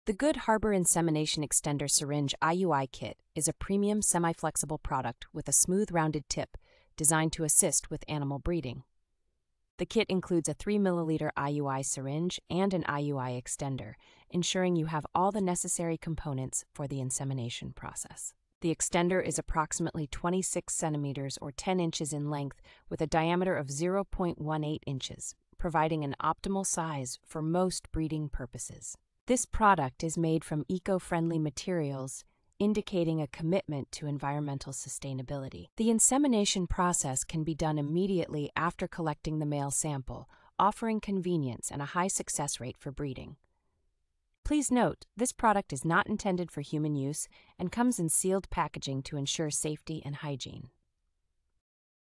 REVIEW (2025): Insemination Extender Syringe IUI Kit. ESSENTIAL details.
The Good|Harbor Insemination Extender Syringe IUI Kit is a premium, semi-flexible product with a smooth rounded tip, designed to assist with animal breeding.

The kit includes a three milliliter IUI Syringe and an IUI Extender, ensuring you have all the necessary components for the insemination process.

The extender is approximately 26 centimeters or 10 inches in length with a diameter of 0.18 inches, providing an optimal size for most breeding purposes.

This product is made from eco-friendly materials, indicating a commitment to environmental sustainability.

The insemination process can be done immediately after collecting the male sample, offering convenience and a high success rate for breeding.

Please note, this product is not intended for human use and comes in sealed packaging to ensure safety and hygiene.